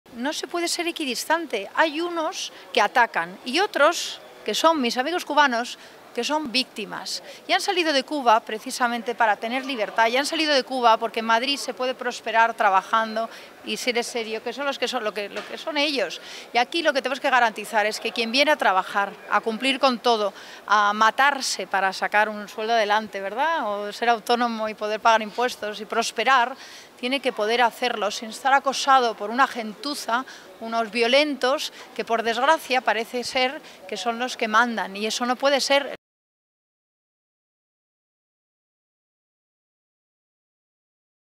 Monasterio sale en defensa de los opositores cubanos del bar atacado en Madrid
Los dueños de un bar ubicado en el barrio de Lavapiés de Madrid han denunciado el ataque que sufrieron el pasado sábado por un grupo de radicales por el mero hecho de ser opositores al régimen cubano. Momento que la diputada en la Asamblea de Madrid y candidata a la presidencia de la Comunidad por Vox, Rocío Monasterio, ha aprovechado para mostrar su apoyo a los comerciantes atacados.